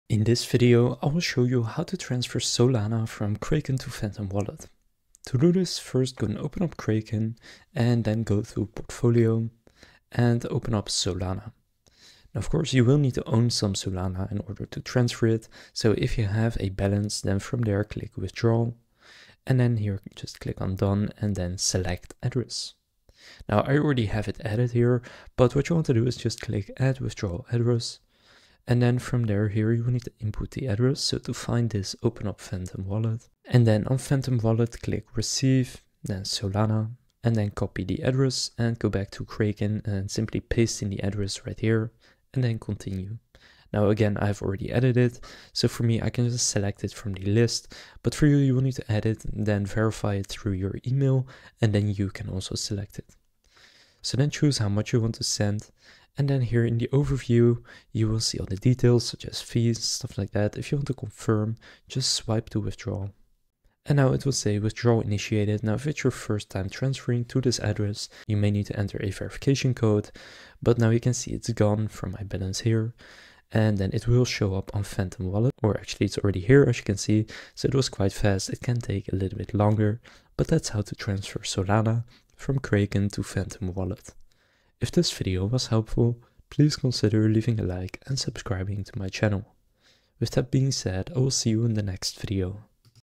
How To Transfer Solana From Kraken To Phantom Wallet - Step by Step
In this video I'll show you How To Transfer Solana From Kraken To Phantom Wallet.